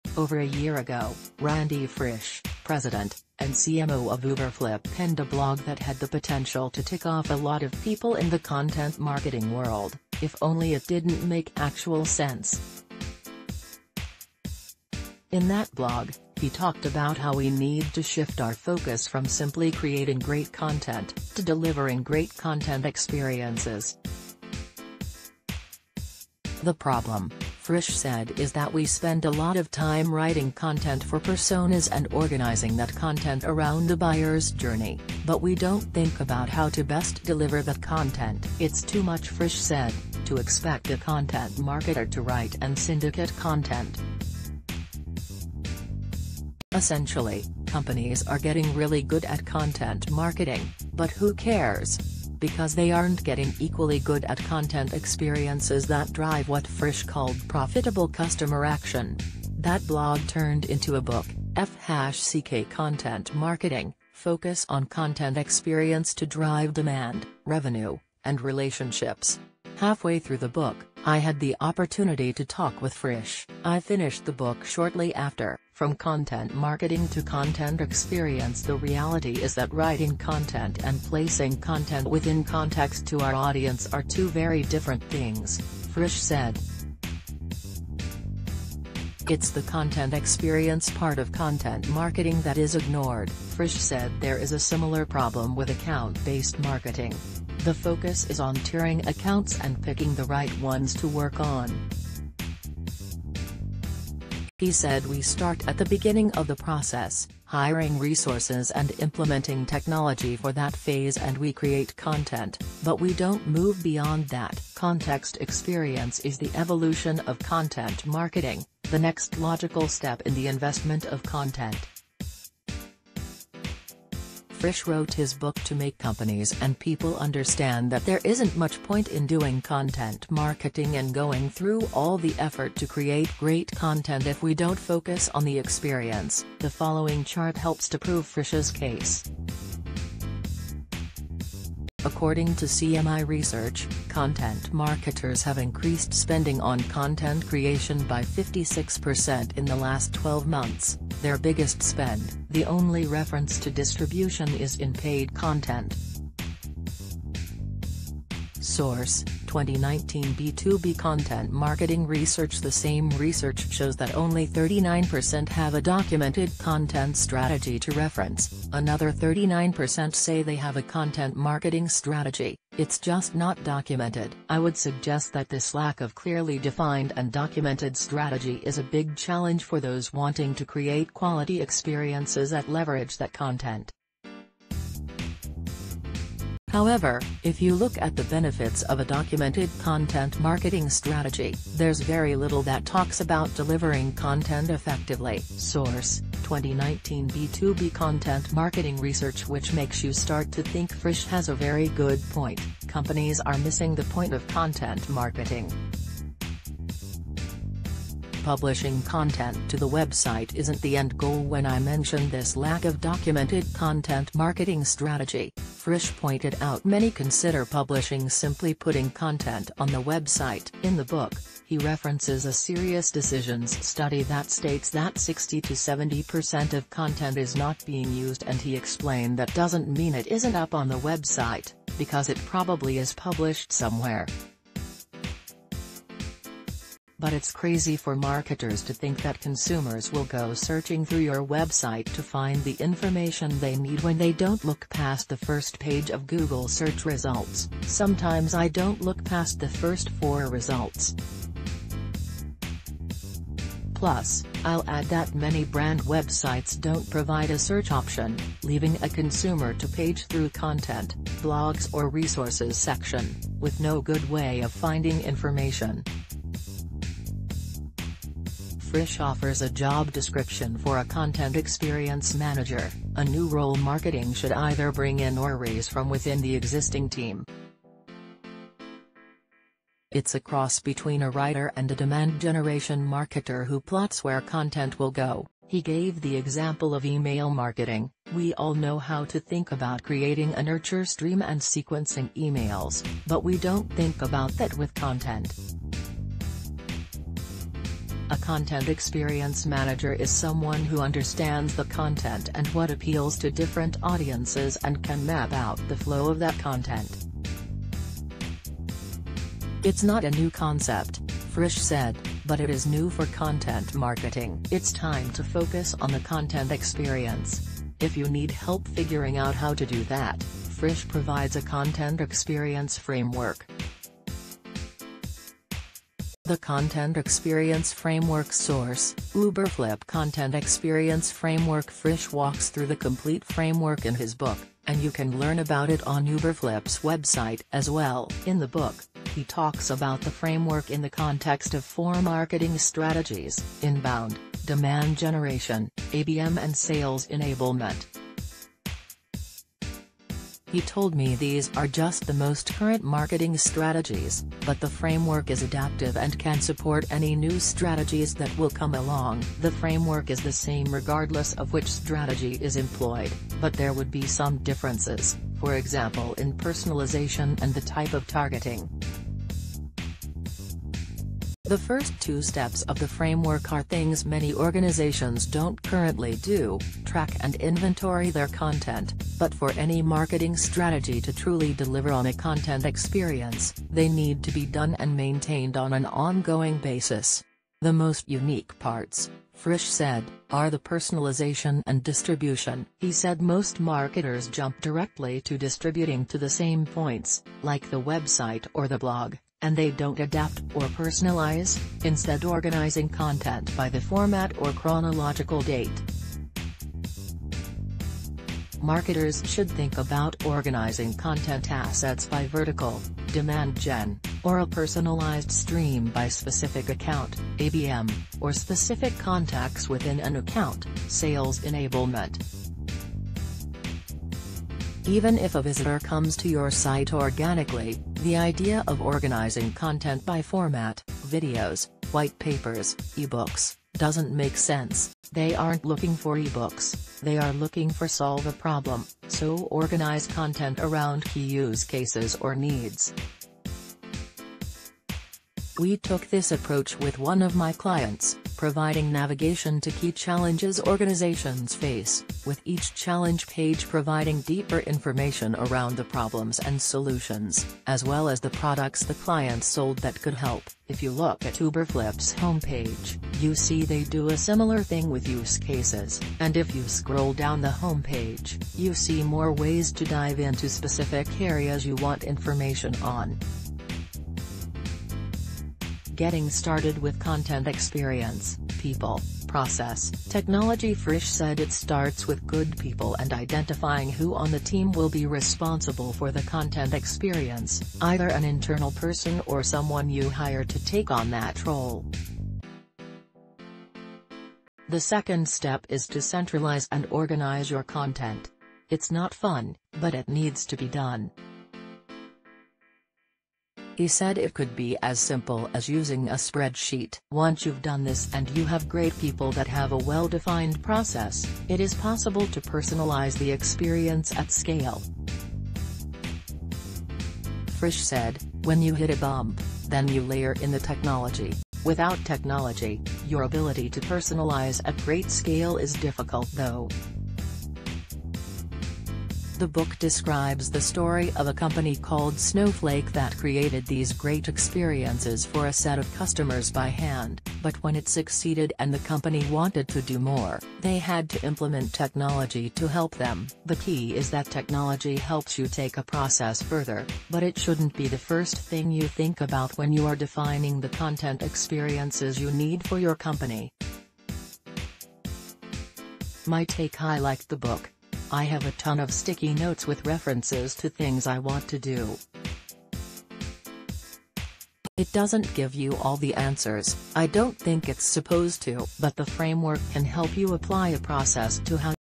Is content experience the evolution of content marketing? | BuzzFresh News
Over a year ago, Randy Frisch, President, and CMO of Uberflip penned a blog that had the potential t...